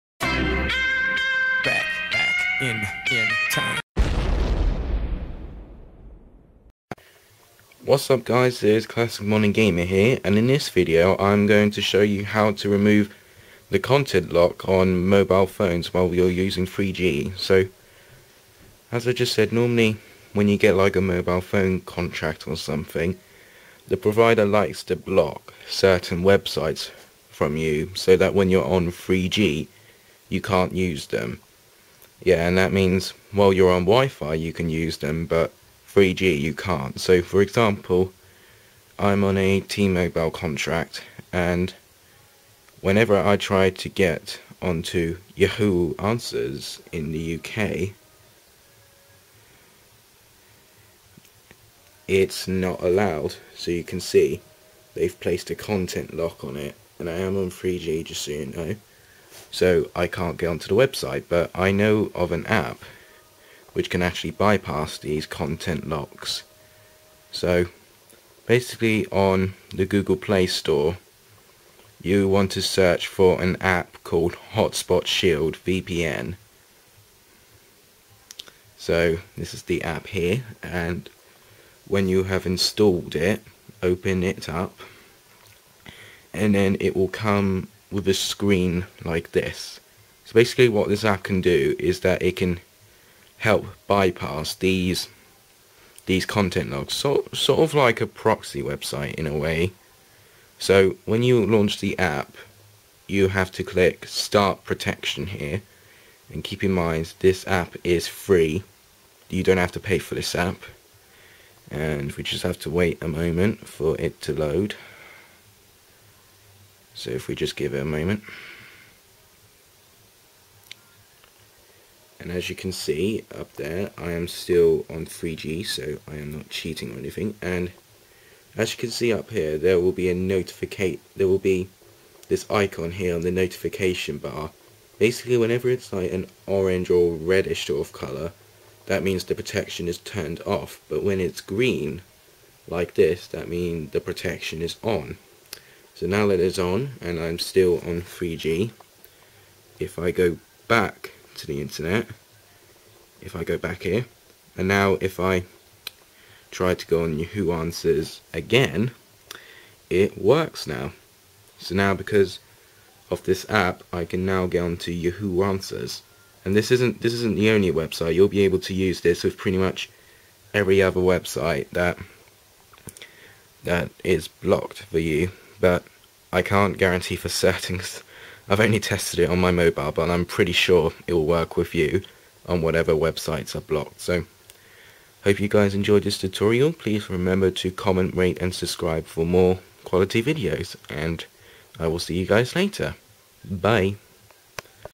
How to bypass content locks on Android
This is an app to help you bypass content locks while on 3G on Android.